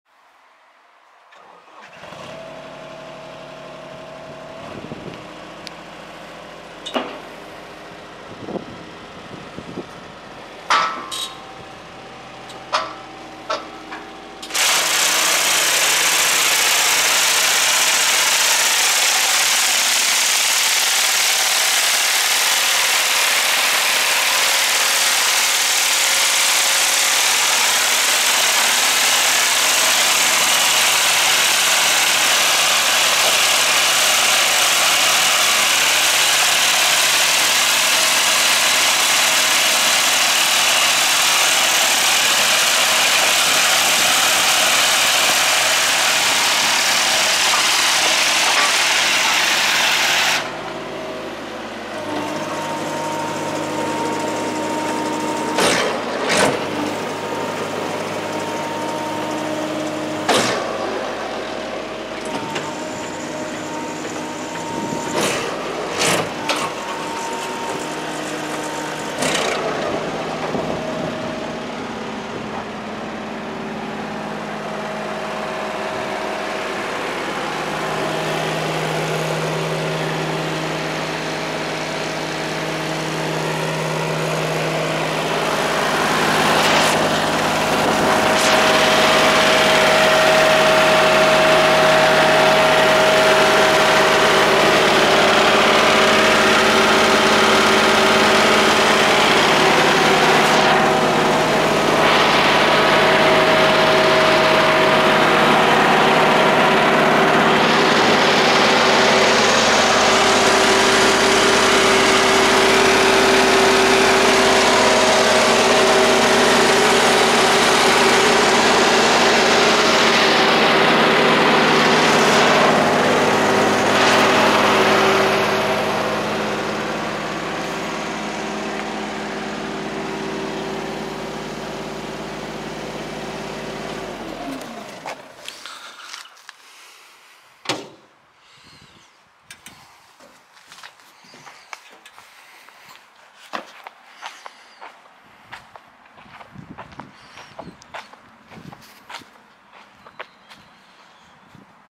Mähboot Aquatic Berkey Gödde Mulag - Mähboot Aquatic Weed Harvester Schneidboot
Mähboot:
- Baujahr geschätzt 2007
- Mähtiefe ca. 1600mm unter Wasser
- Mähbreite ca. 2000mm
- rechts-links schwenkbar
- Schaufelradantrieb
- Frontlader mit Mähwerk
- 3 Zylinder Lombardini Motor LDW1603 37,5PS, 1649ccm
- 1 Sitz
- Slip-Anhänger (el. Winde und hydraulisch kippbar)
- Mähstunden 1056
- Motorstunden 1273

90530 Wendelstein